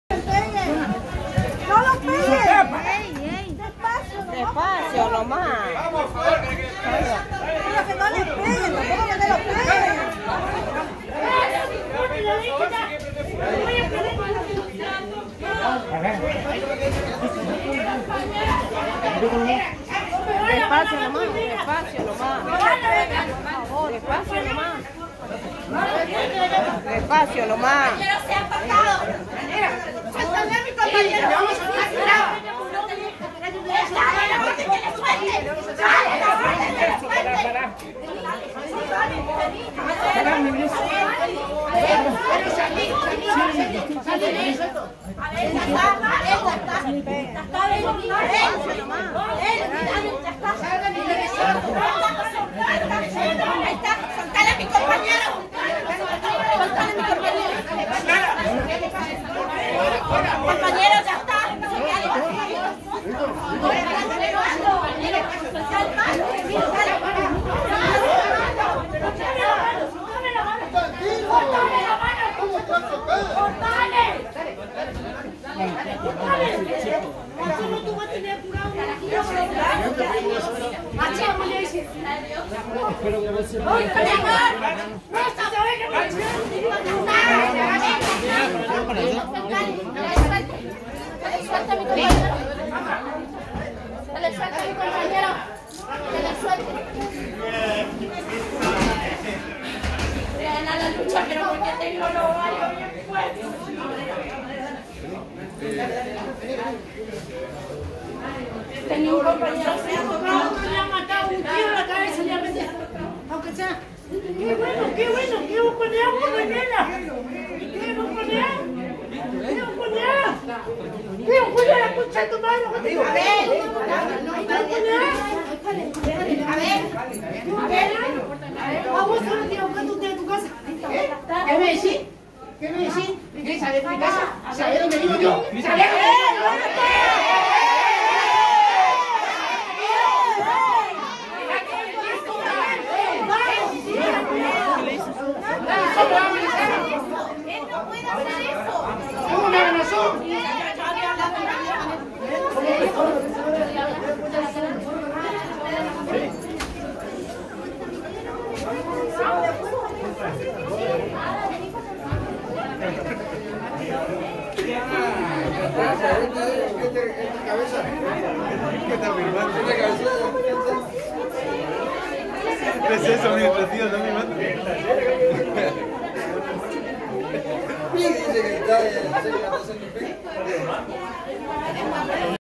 Tensión en el 8°piso de Casa de Gobierno de Chaco
Empleados del sector amenazaron con prenderse fuego e inciendiar el octavo piso de Casa de Gobierno - Chaco. Para que sus reclamos sean atendidos por el Gobernador Capitanich en el tema: "Pase a Planta" personal contratado.